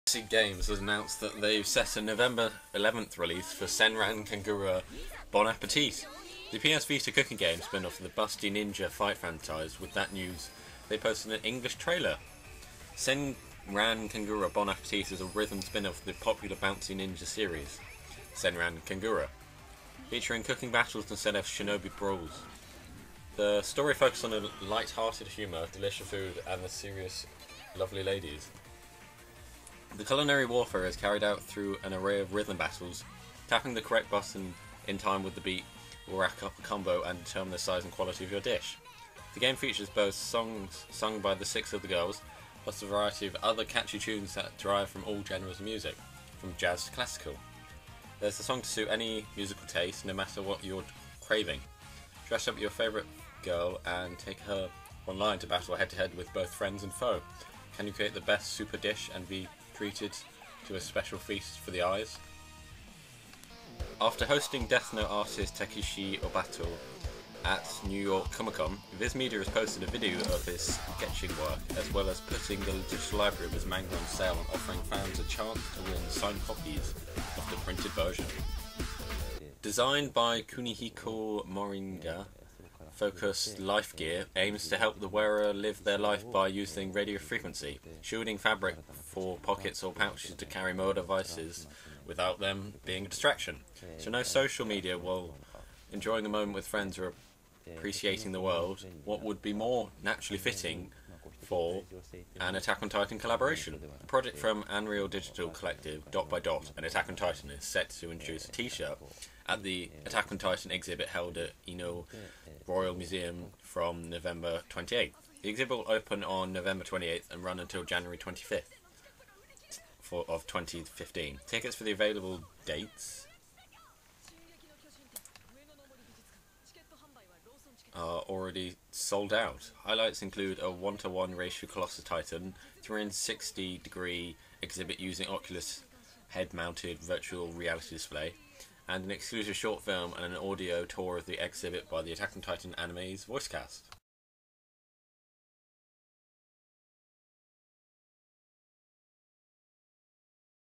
Anime News: Senran Kagura, Death Note Artist & Attack on Titan Fashion
English Senran Kagura: Bon Appétit! Spin-Off Game Trailer. Death Note Artist Takeshi Obata Sketches at New York Comic Con. Attack on Titan Teams Up With Mobile Tech Blocking Fashion.